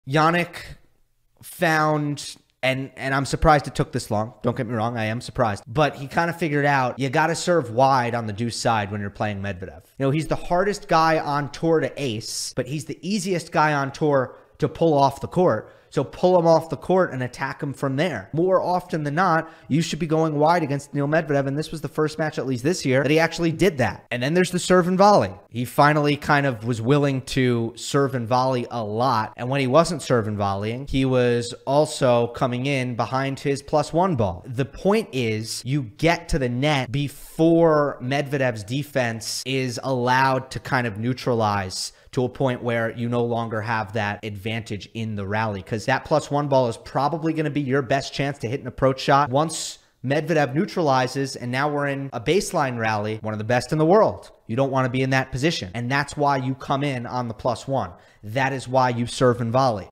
Sinner beats Medvedev with SMART serve changes
Jannik Sinner beat Daniil Medvedev to win the China Open this week in Beijing. Sinner finally made some key changes to his serving tactics and they worked in his favor. Between serving out wide and serving and volleying, Sinner kept to his plan and came out on top.

Enjoyed the video? Become a member to ensure the long-term future of the channel:

Gill Gross provides in-depth ATP tennis analysis. Match breakdowns, draw previews, the latest news, and other tennis insights...